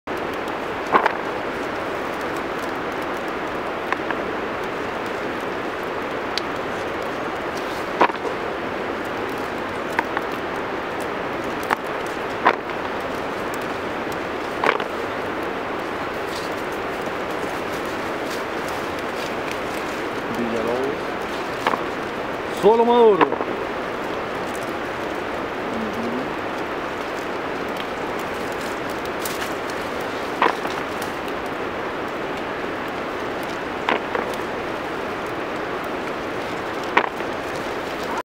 Harvesting Down to Earth Villalobos varietal coffee in Providencia de Dota, Tarrazu region
This is a video to show both the first harvest of our Villalobos coffee varietal and the technique necessary to only pick the red, ripe beans.  Johnny is our best Guaymi picker and he was assigned exclusively to do the Villalobos.  The Guaymi pickers are native Panamaian migrant workers who are essential to our process because they are very diligent and respect our need of only ripe beans.